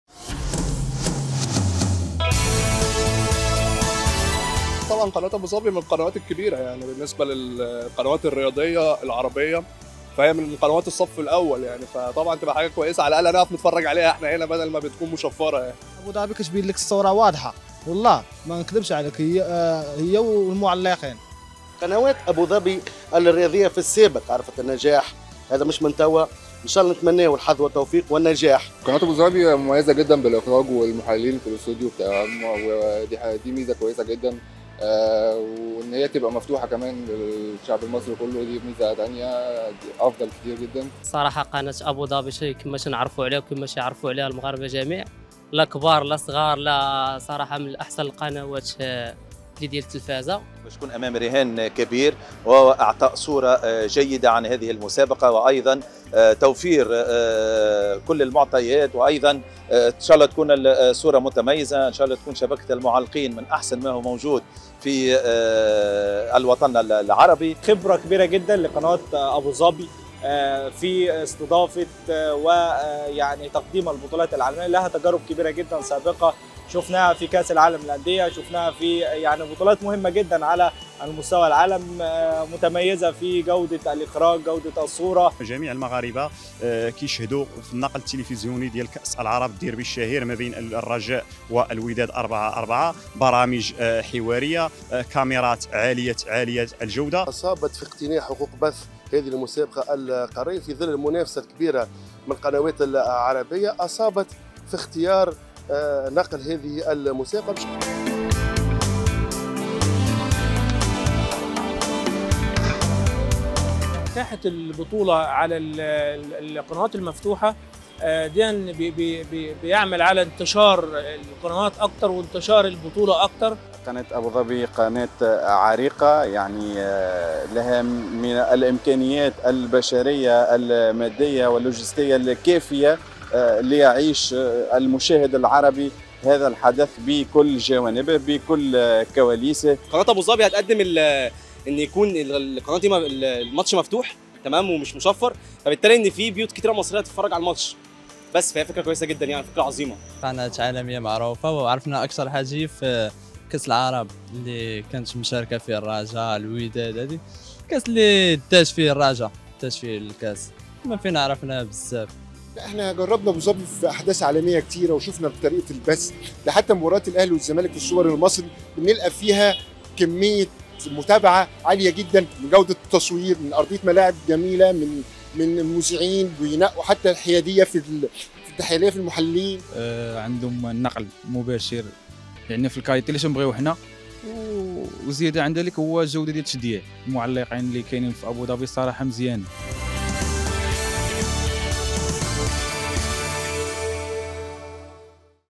أبوظبي الرياضية تحصل على حقوق بث الدوري الأفريقي - ردود الأفعال
الدوري الأفريقي (السوبر ليج) على قنوات أبوظبي الرياضية المفتوحة ومجاناً